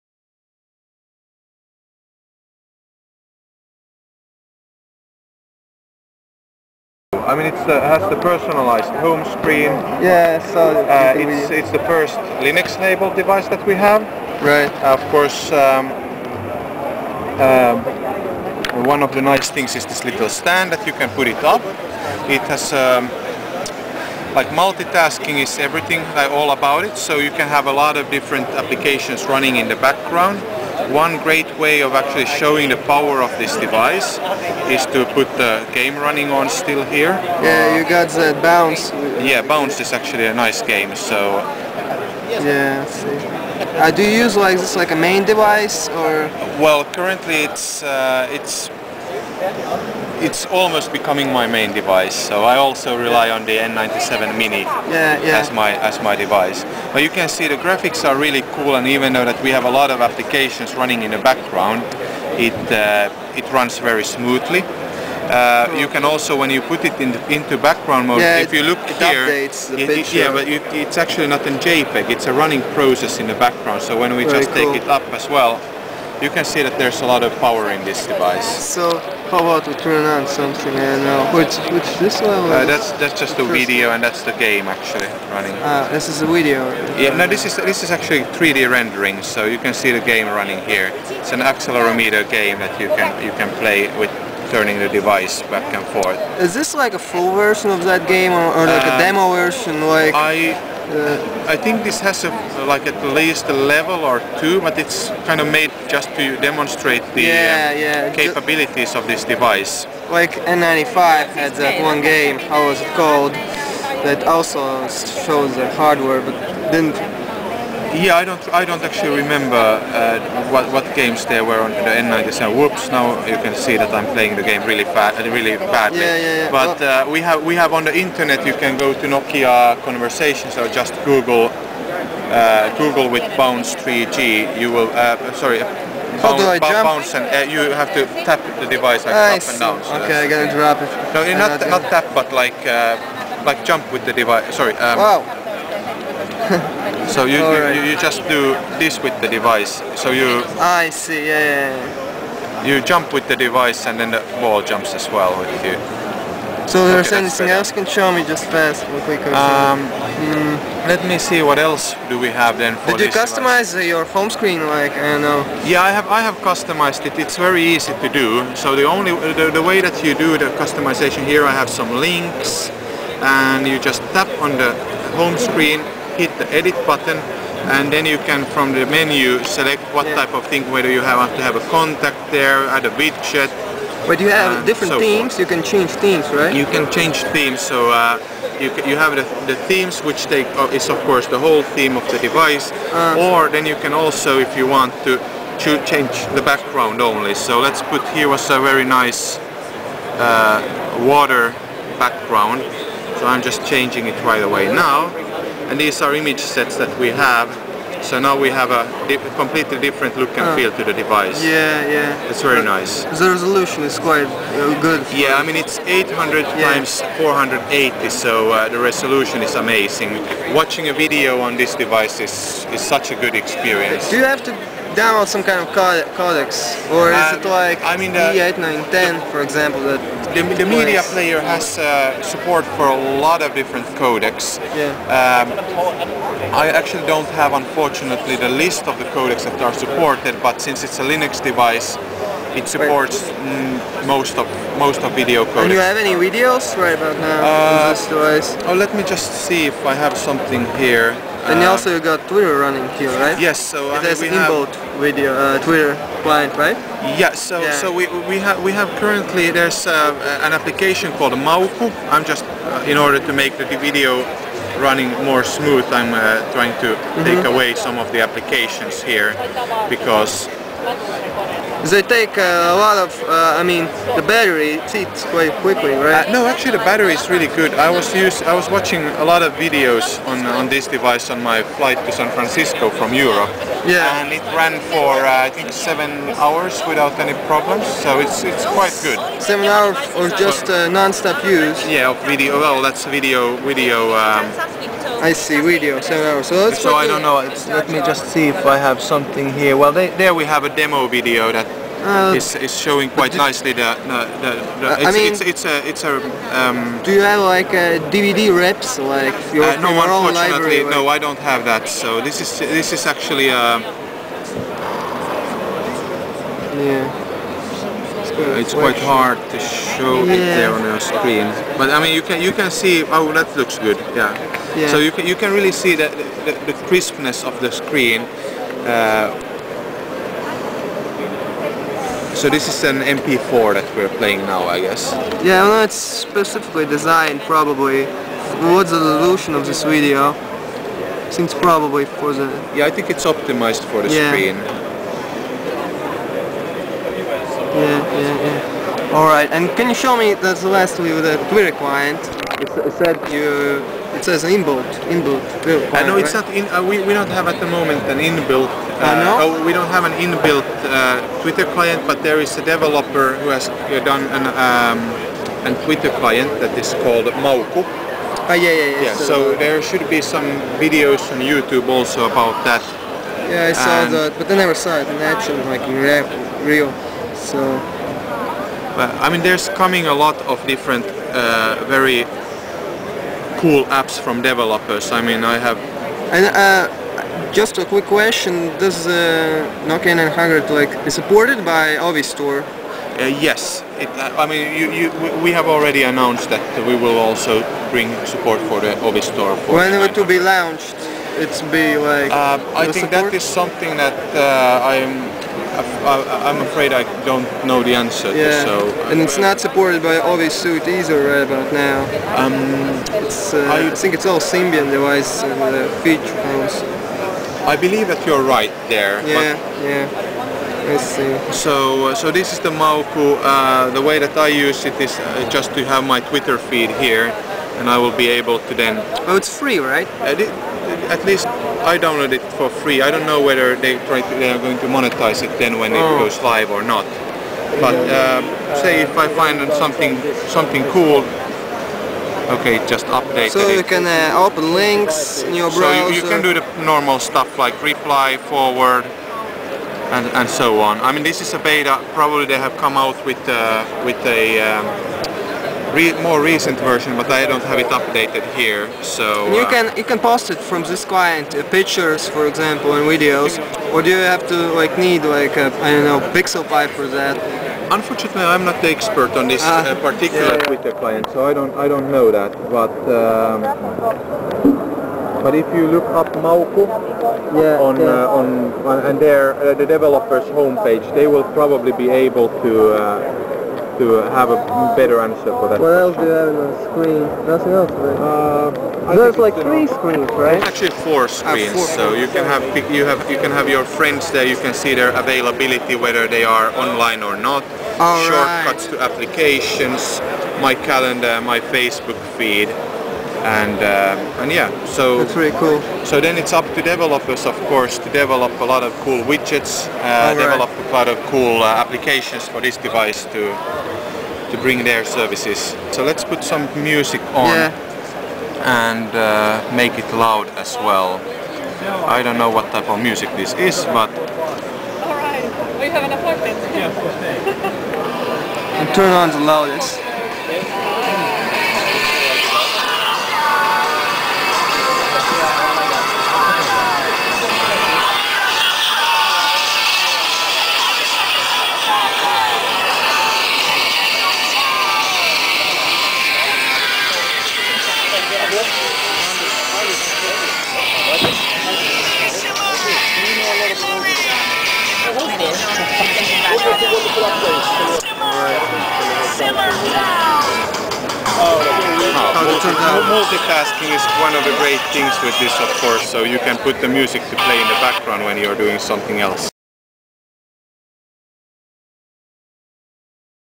Nokia N900 quick walkthrough
A quick tour around the main attractions of the upcoming Maemo OS based Nokia mobile computer!